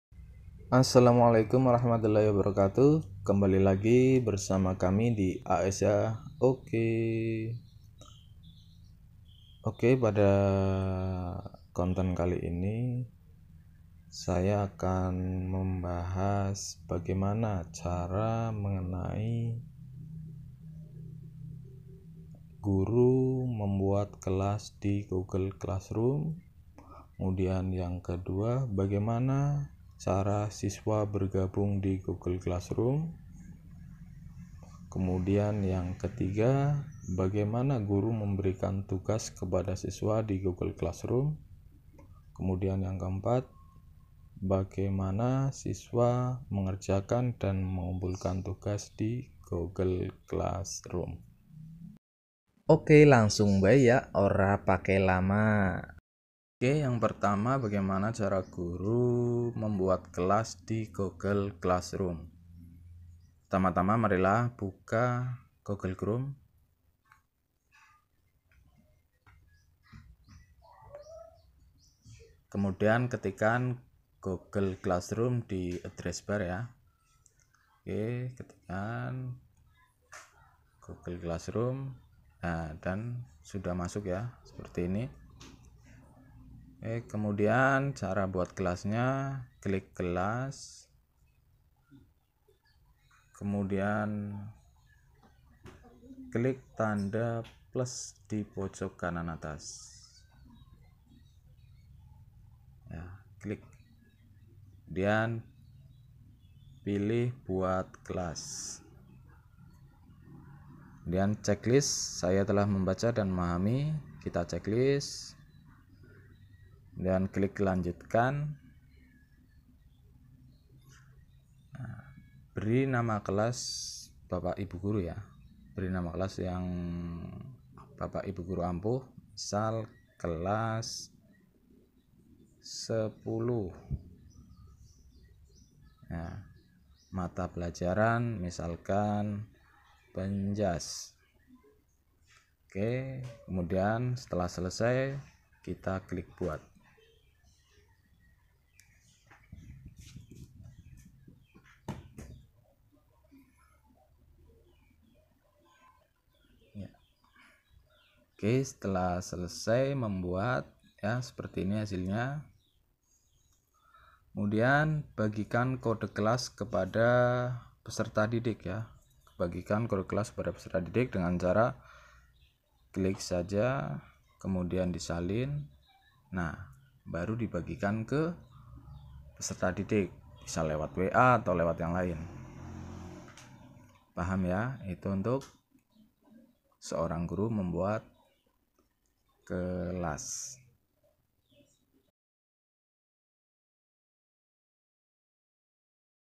Cara Guru Membuat Kelas Di Google Classroom
Di tengah pandemi corona. Pembelajaran yang dilakukan seorang guru pun melalui pembelajaran daring (online) dari situ guru juga harus pintar-pintar memilih cara bagaimana pembelajaran daring tersebut dapat berjalan efektif dan lancar. Nah disini saya sendiri menggunakan google classroom karena lebih efektif dan mudah digunakan. Penyimpanan tugas pun bisa langsung tersimpan di drive. Kalau guru harus menggunakan Whatsapp dalam pengiriman tugas, bisa-bisa memori penuh karena download tugas siswa.
Untuk tutorial berikutnya ditunggu ya. Semoga bermanfaat. Maaf apabila banyak kekurangan saat penyampaian ataupun memberikan penyajian. Karena memang saya sendiri masih belajar. Terima kasih.